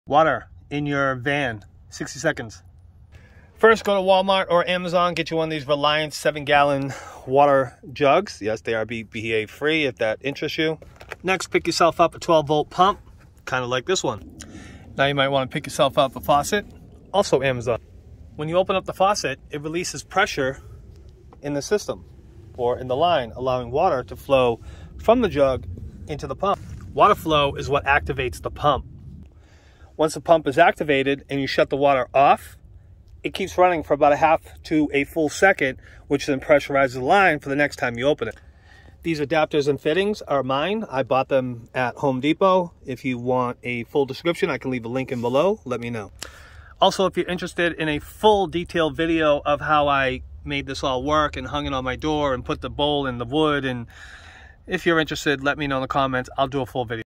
Vanlife a simple water and sink in my van.
how to have a sink in a van. this is my simple setup which allows me to still open my door fully. if you want a full video. let me know. I'll do one 🕐